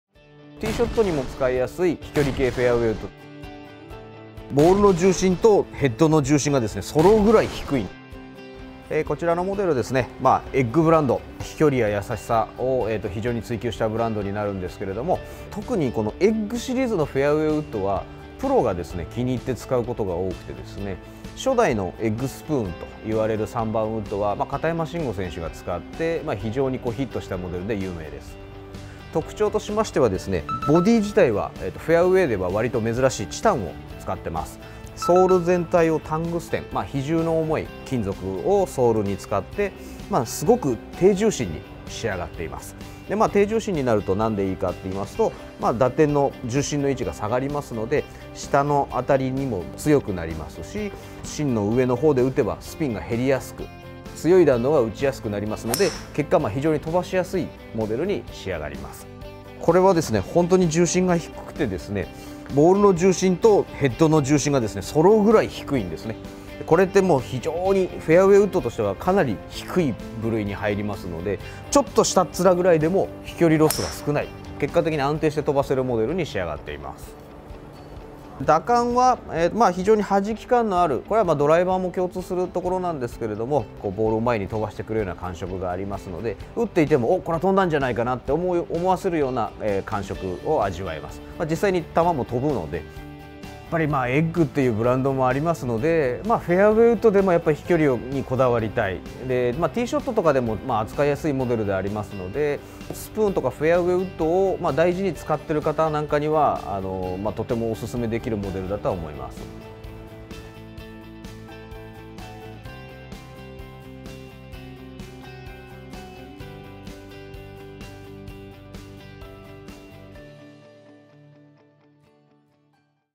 【試打】ティーショットでも使える飛距離系！プロギア「NEW egg フェアウェイウッド」
後発メーカーならではの発想で、従来のゴルフ業界には無い革新的な製品を作り出しているプロギア。中でも、eggシリーズのフェアウェイウッドはプロの間でも人気のブランドです。そこで今回は、ゴルフフィッターの小倉勇人さんがプロギア「New egg フェアウェイウッド」を試打して、その特徴を解説していただきました。

※本動画はスポーツナビが独自で企画したものです。動画内の商品選定や評価はスポーツナビまたは出演者の方の判断にもとづいています。動画内で使用している商品画像はメーカーから画像・サンプルをお借りした上で使用、撮影しています。